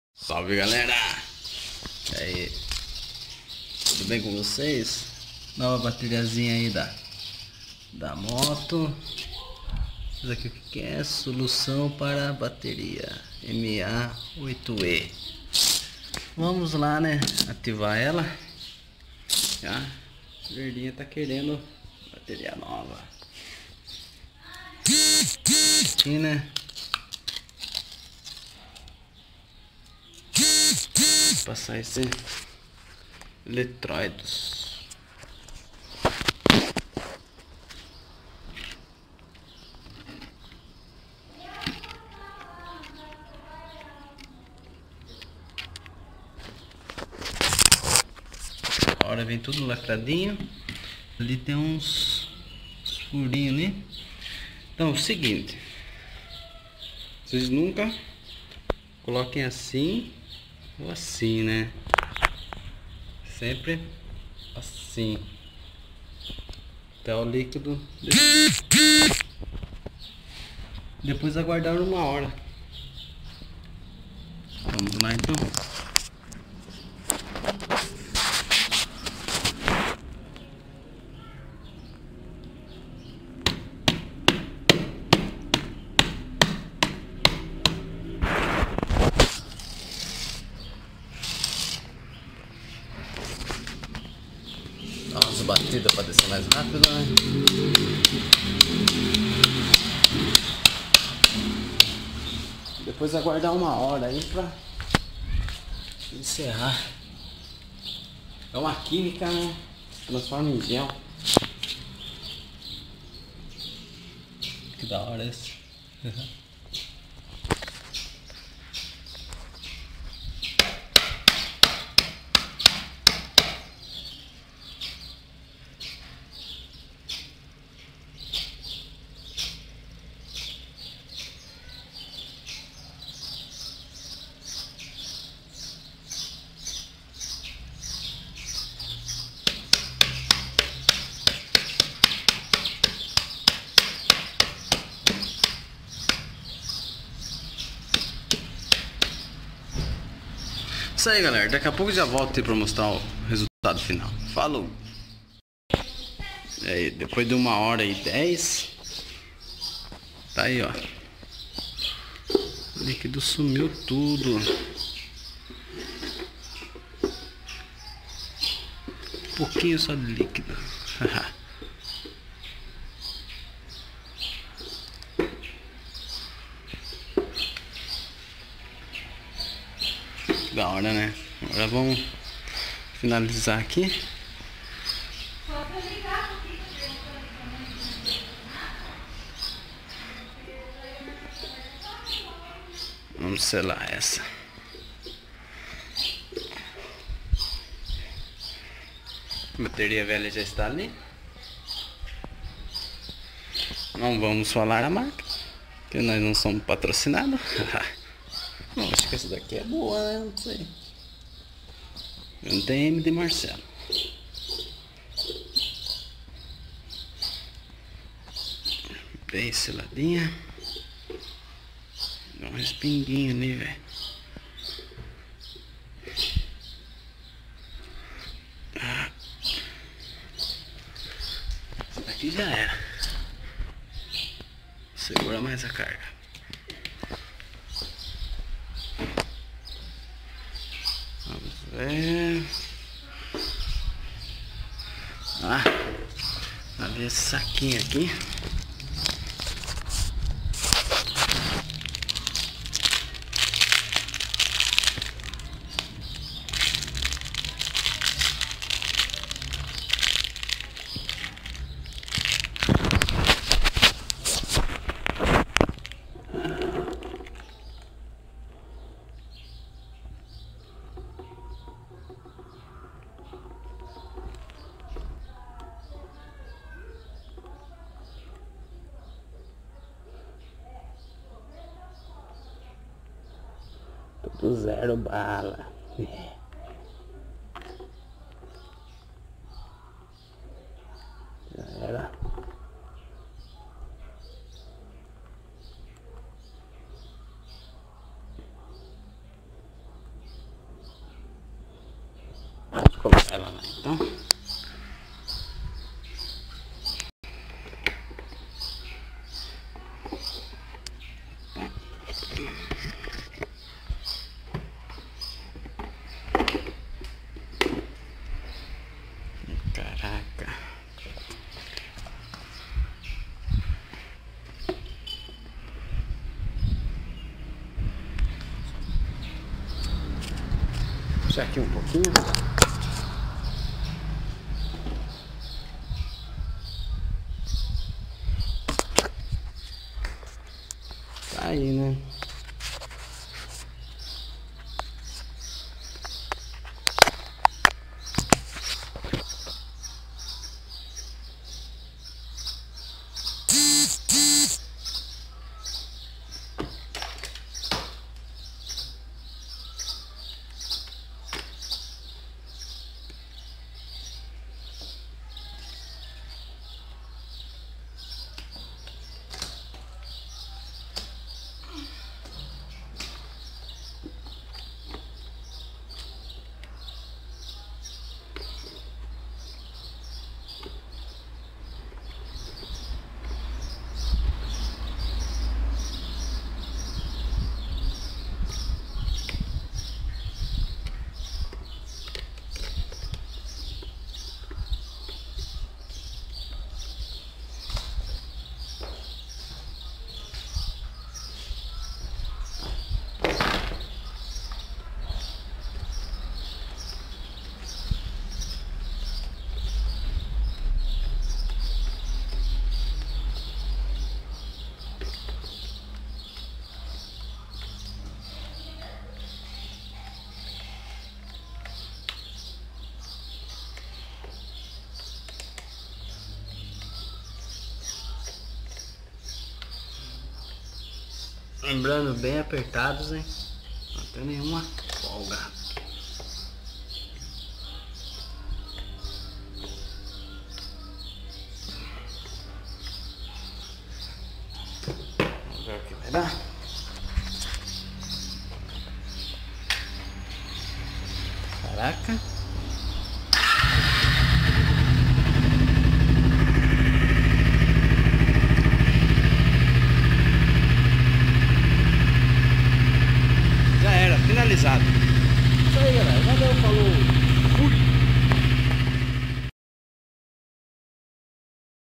MR021 Como fazer a ativação da bateria para moto
Salve glr quem gostou curte ai, tmj
Uma breve como fazer a ativação da bateria moura, simple e facil de fazer, pode fazer vc mesmo na casa.